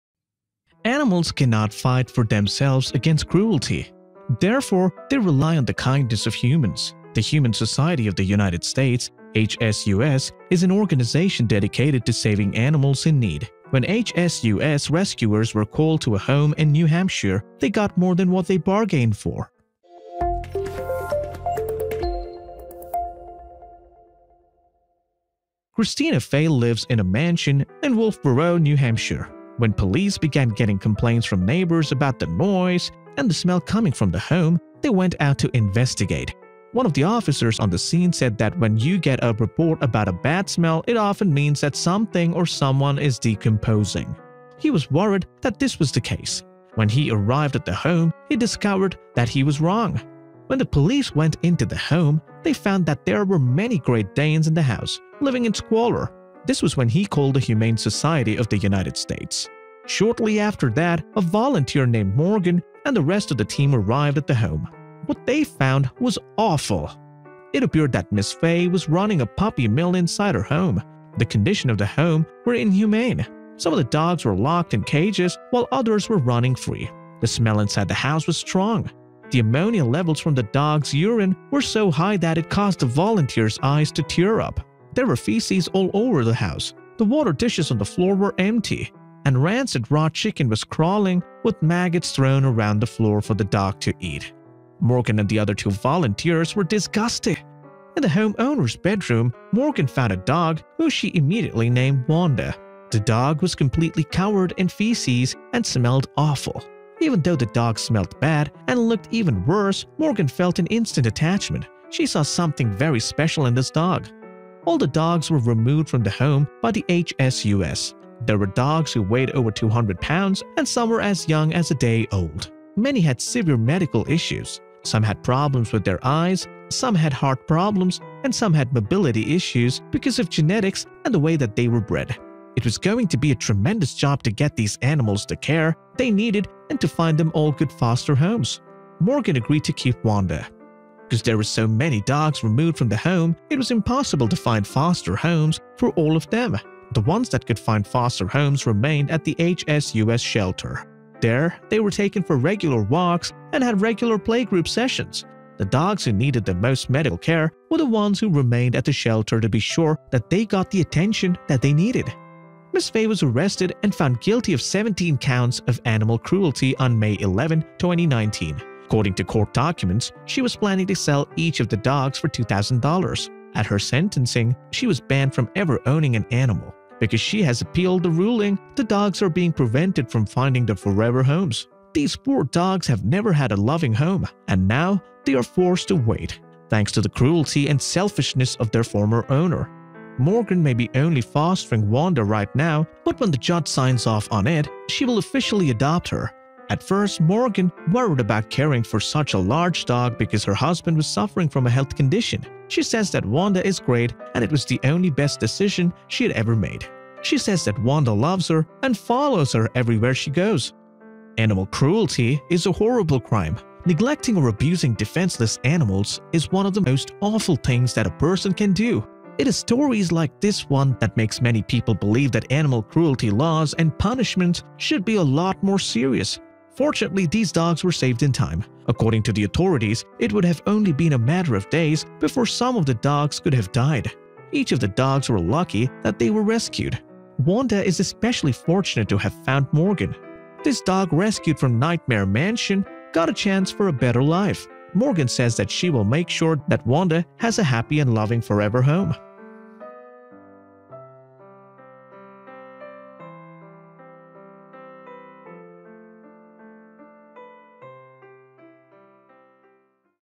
Dog Rescued From “Nightmare Mansion” Got A Chance For A Better Life
Dog Rescued From “Nightmare Mansion” Got A Chance For A Better Life.When the police went into the home, they found that there were many Great Danes in the house, living in squalor. This was when they called the Humane Society . Dog Rescued From “Nightmare Mansion” Got a Chance for a Better Life. When rescuers opened the mansion's front door, they were stunned by .

Animals cannot fight for themselves against cruelty; therefore, they rely on the kindness of humans. The Humane Society of the United States (HSUS) is an organization dedicated to saving animals in need. When HSUS rescuers were called to a home in New Hampshire, they got more than they bargained for.